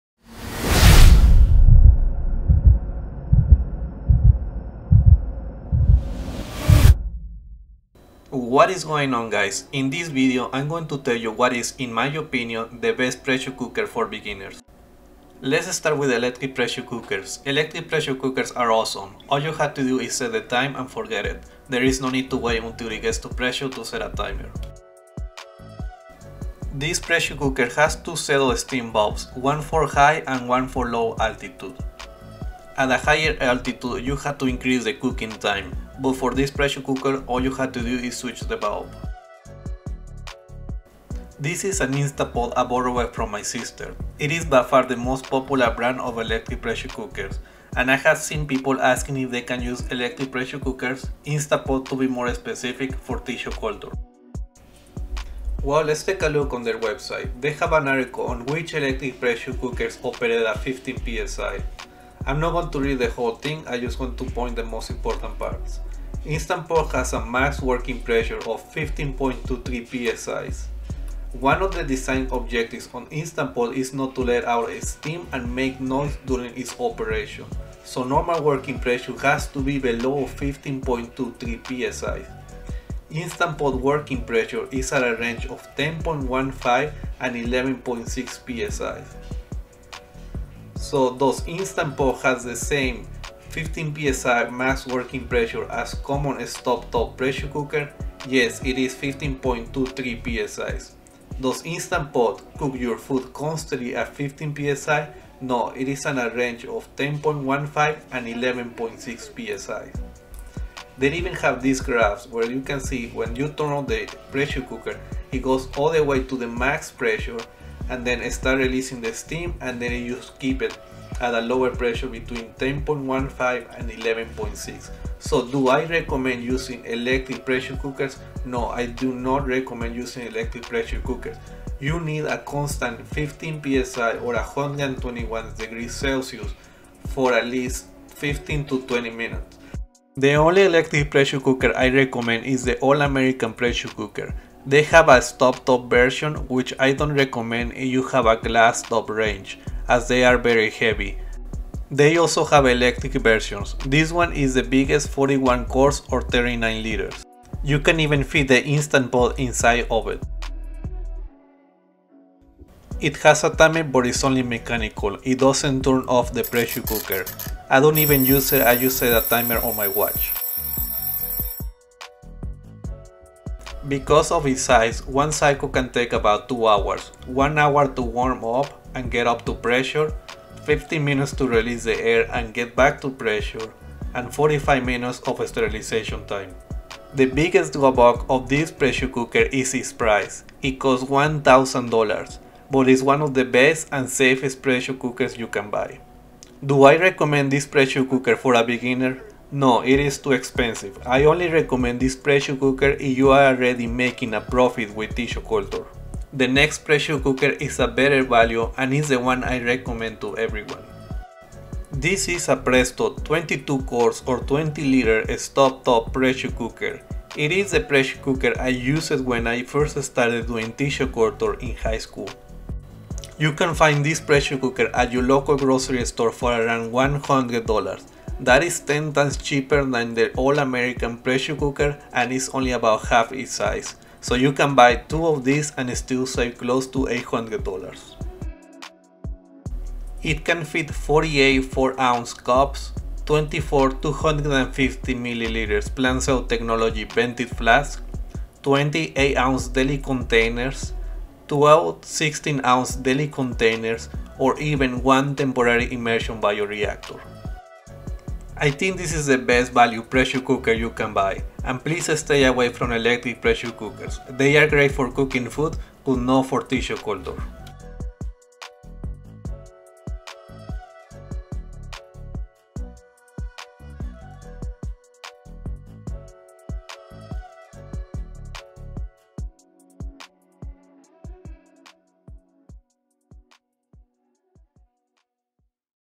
Best Pressure Cooker For Tissue Culture
Autoclaving the equipments used in tissue culture is a crucial step. But, if you have homemade tissue culture space then the professional autoclaves will be very pricey for you. The best alternative is pressure cooker, but not all cookers fits your purpose. So, in this video we are going to tell you which pressure cooker should be avoided to use and which one will be best for beginners and small to medium businesses.

Are you throwing away your contaminated explants? Don't do that! Because you can re-sterilize them and use them again that will save your time and money put in the culturring processes. So, in this video we are going to reuse one of our contaminated culture of Mostera deliciosa and show you the re-sterilization process by using its roots.

We are Plant Cell Technology. We create innovative solutions for commercial and research plant tissue culture laboratories and growers.